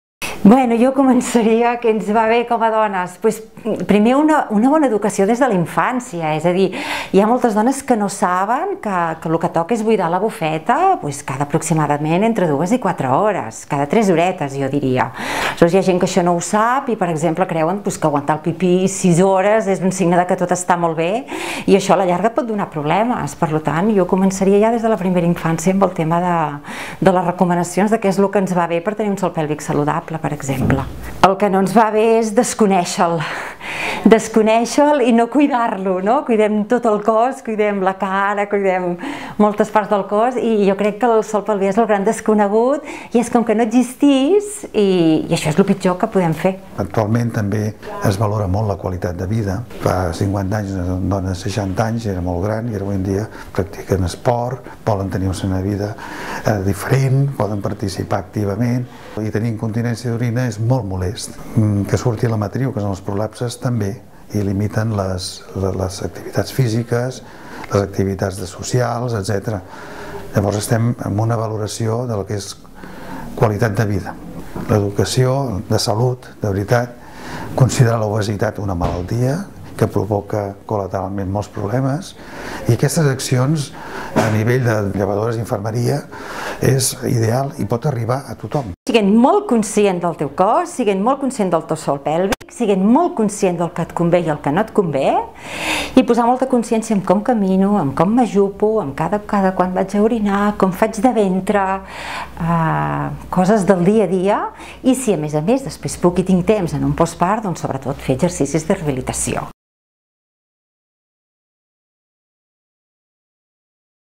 L'educació en salut, el tractament més efectiu
Tant el Josep Grau com la Montse Verdaguer coincideixen que una bona educació, des d'edats primerenques, és clau per prevenir lesions del sòl pelvià. Com bé explica la llevadora, "el problema de les patologies del sòl pelvià és que la gent no consulta fins que la disfunció és greu. El més bàsic és educar per prevenir i actuar així que aparegui un mínim problema". Per això, ella creu que per mantenir un sòl pelvià fort cal començar per la prevenció, saber què ens va bé i què ens perjudica i rehabilitar-lo com més aviat millor.